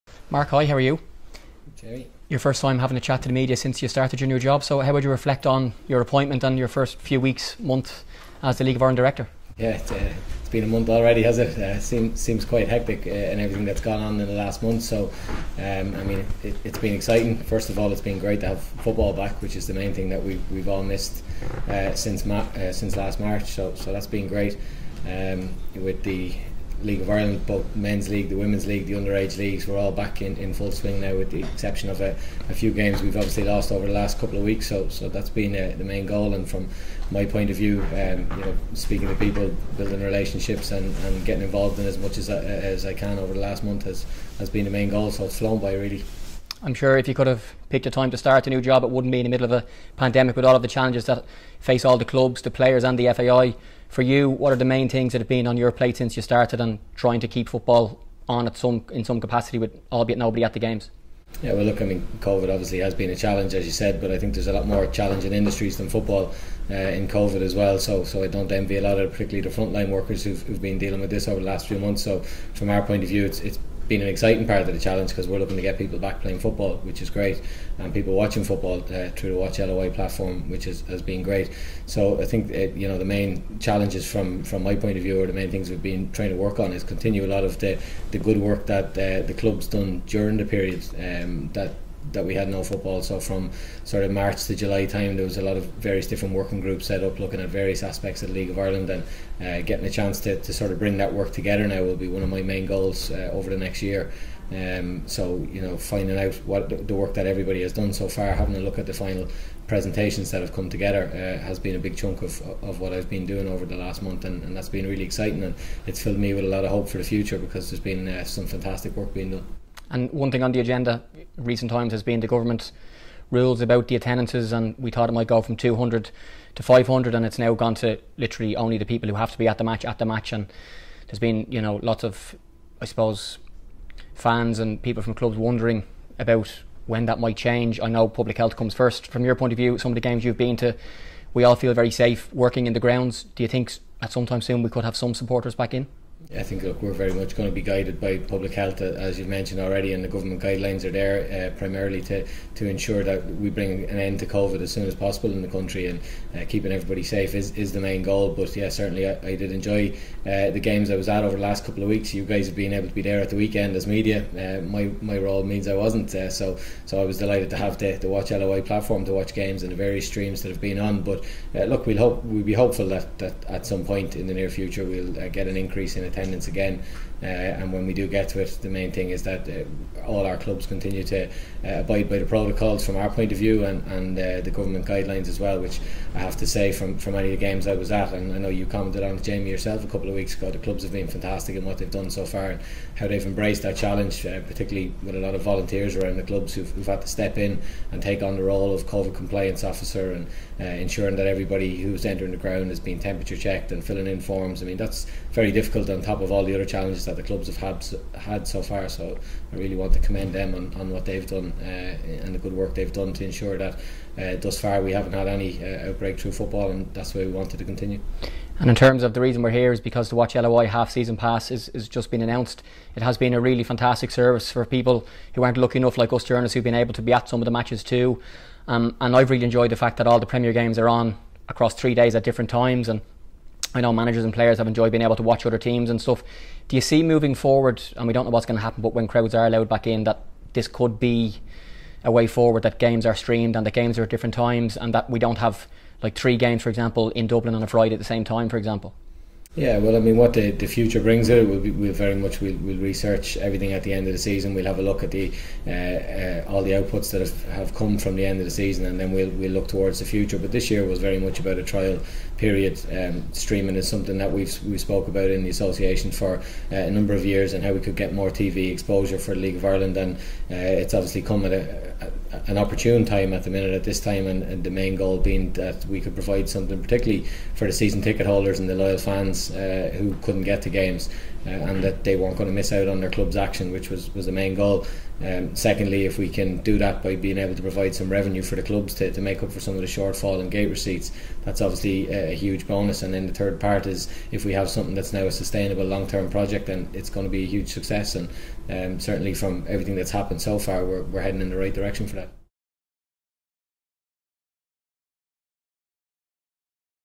New League Of Ireland Director Mark Scanlon’s First Interview
New SSE Airtricity League Director Mark Scanlon has spoken to the media for the first time since starting ‬

‪I asked him how his few first weeks have been & about the main challenges he has faced running the the LOI in the middle of a global pandemic, about WATCHLOI service‬ & more